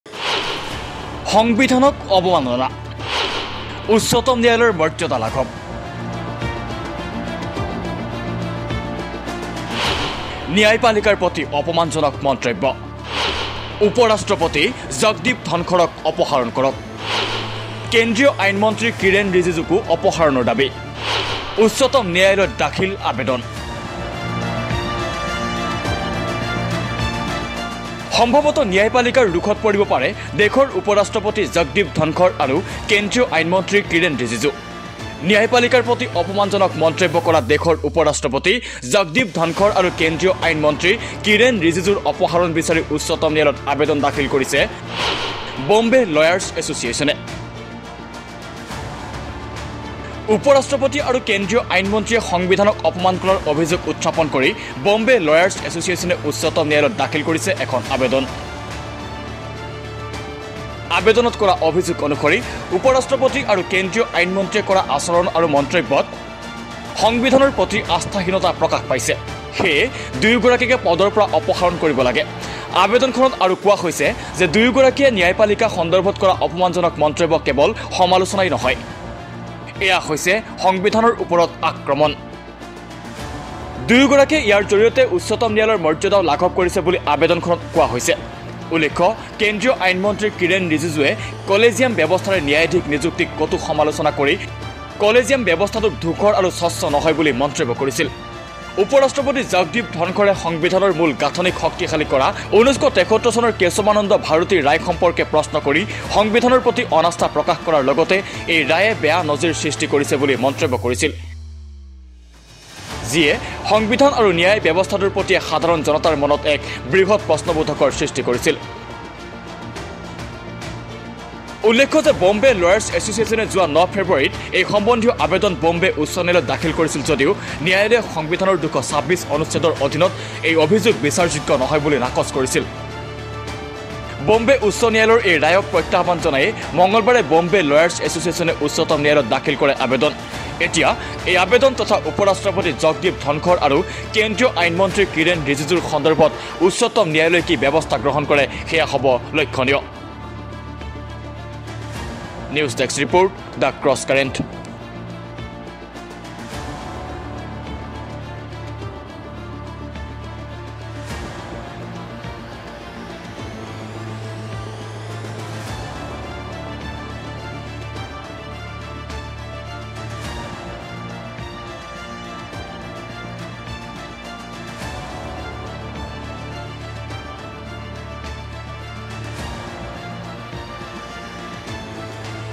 জগদ্বীপ ধনখড় আৰু কিৰেন ৰিজিজুক অপসাৰণৰ দাবী ৷ কিন্তু কিয় ?
সংবিধানক অৱমাননা ৷ উচ্চতম ন্যায়ালয়ৰ মৰ্যাদা লাঘৱ৷ ন্যায়পালিকাৰ প্ৰতি অপমানজনক মন্তব্য ৷
উপ-ৰাষ্ট্ৰপতি জগদ্বীপ ধনখড় আৰু কেন্দ্ৰীয় আইন মন্ত্ৰী কিৰেন ৰিজিজুক অপসাৰণৰ দাবী ৷